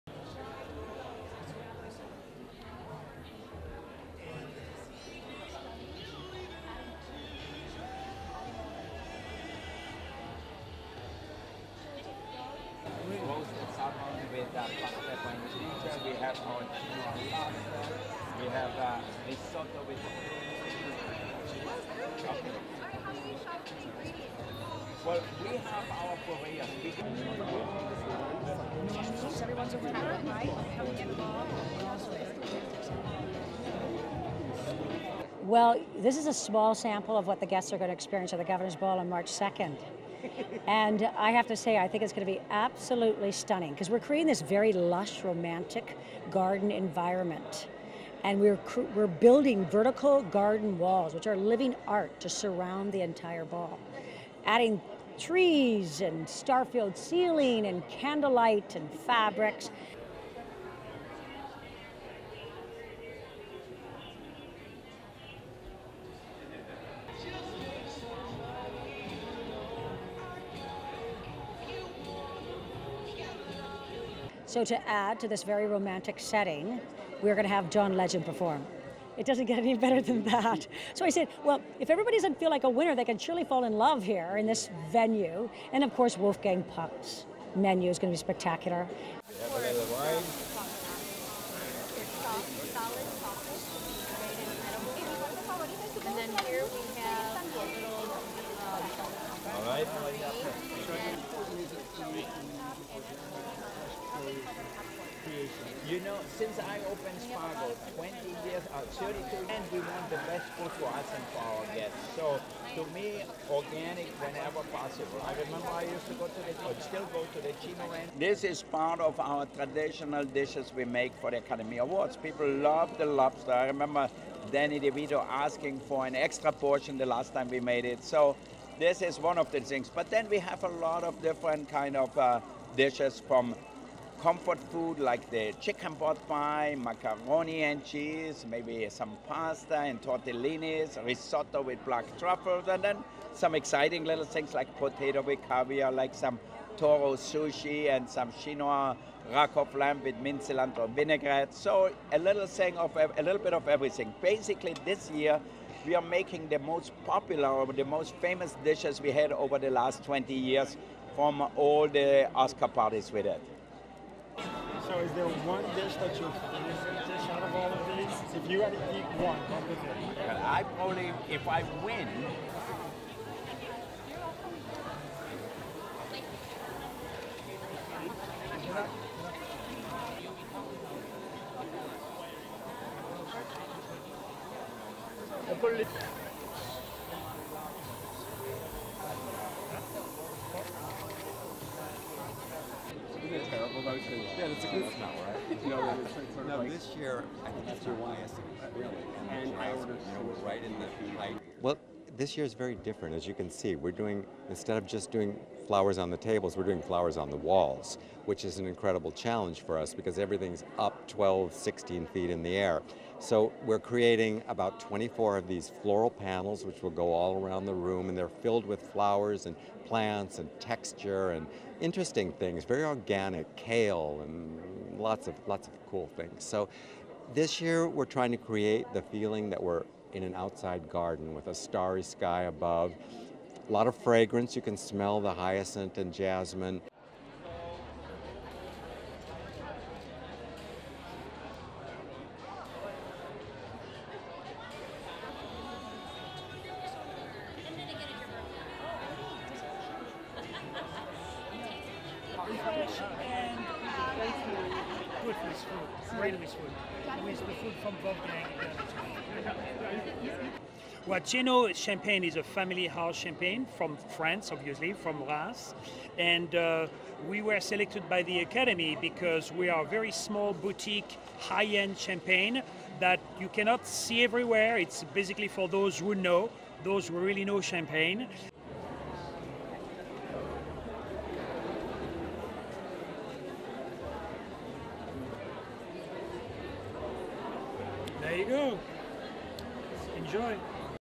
A preview of the food and decor at the Governors Ball, the Academy's official Oscar after party
(21 Feb 2014) PREPARATIONS UNDERWAY FOR HOLLYWOOD'S BIGGEST AFTER PARTY
It's champagne wishes and caviar dreams at the annual Governors Ball, the official post-Oscars bash.
To be exact, there will be 5 kilos of farm-raised caviar and 1,350 bottles of Thienot champagne at the celebration hosted by the Academy's board of governors.
The elegant soiree takes place in the ballroom of the Hollywood and Highland center - about 50 feet from the site of the Oscars ceremony, the Dolby Theater. And while the Oscars ceremony itself has some 3000 attendees, only about 1500 special guests receive invites to the ball.
"Well this is a small sample of what the guests are going to experience at the Governors Ball on March 2nd. And I have to say, I think it's going to be absolutely stunning because we are creating this very lush, romantic garden environment and we're building vertical garden walls, which are living art, to surround the entire ball, adding trees and a star-filled ceiling and candlelight and fabrics," said the event producer Cheryl Cecchetto.
John Legend will serenade the A-List crowd while famed chef Wolfgang Puck will whip up a feast fit for a king.
The menu includes spicy tuna tartare, mini Kobe burgers, "smoked salmon Oscar matzo," Taro root tacos, chicken pot pie and homemade pretzels.
"This is part of our traditional dishes we make for the Academy Awards. People love the lobster. I remember Danny DeVito asking for an extra portion the last time we made it. So this is one of the things, but then we have a lot of different kinds of dishes from comfort foods like chicken pot pie, macaroni and cheese maybe some pasta, and tortellini's, to risotto with black truffles. And then some exciting little things like potato with caviar, like some taro sushi and some rack of lamb with mint cilantro vinaigrette. So a little bit of everything. Basically this year we are making the most popular or the most famous dishes we have had over the last 20 years from all the Oscar parties we did," said Puck, who has catered the exclusive event for the last 20 years.
Dinner will include 6,500 Oscar-shaped flatbreads, 250 pounds of troll-caught Atlantic Bigeye, 1,300 oysters, 600 Maine lobsters and 10 pounds of truffles from Burgundy, France.
Floor to ceiling floral arrangements by florist Mark Held will create a romantic garden party feel.
"This year is very different as you can see. We're doing, instead of just doing flowers on the tables, we are doing flowers on the walls, which is an incredible challenge for us because everything is up 12, 16 feet in the air. So we are creating about 24 of these floral panels, which will go all over the room and they're filled with flowers and plants and texture and interesting things, very organic, kale and lots of cool things. So this year we are trying to create the feeling that we're in an outside garden with the starry sky above, a lot of fragrance. You can smell the hyacinth, the jasmine," said Held.
Guests will sip champagne from Thienot, a small boutique in Reims, France. There are only 250,000 bottles of the exclusive bubbly available in the world.
"Thienot is a family house champagne from France obviously, from Reims, and we were selected by the Academy because we are very small boutique, high-end champagne that you cannot see everywhere. It's basically for those who know, those who really know champagne," said Fran�ois Peltereau-Villeneuve, President and CEO of Thienot USA.
The 86th annual Academy Awards will air live on Sunday, March 2 on ABC.